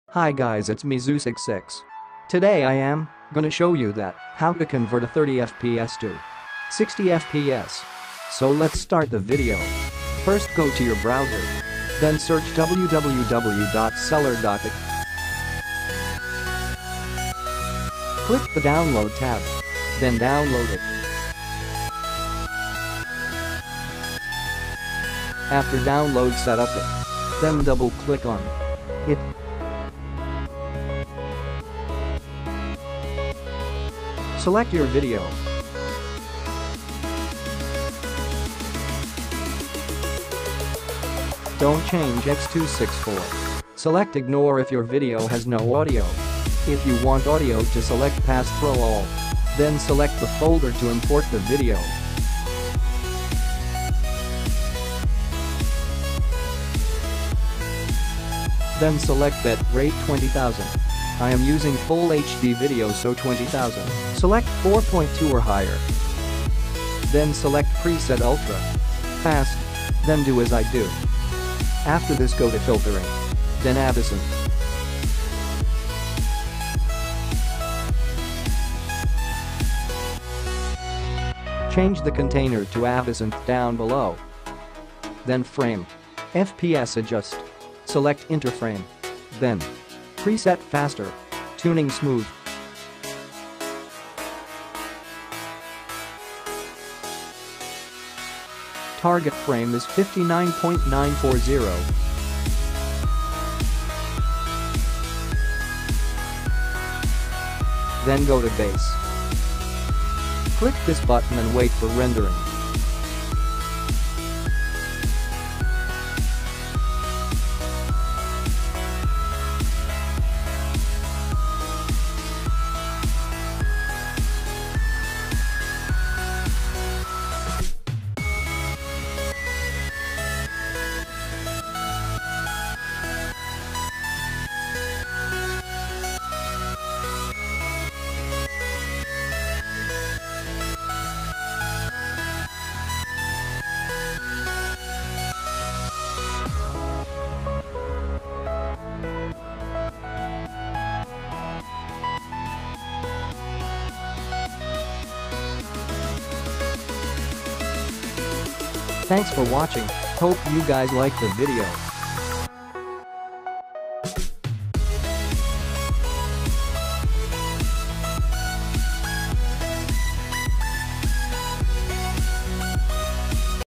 Convert any video into 60fps
0:00Intro😎
 0:07installing hybrid
 0:28How to convert into 60fps
 2:41 60fps video

etc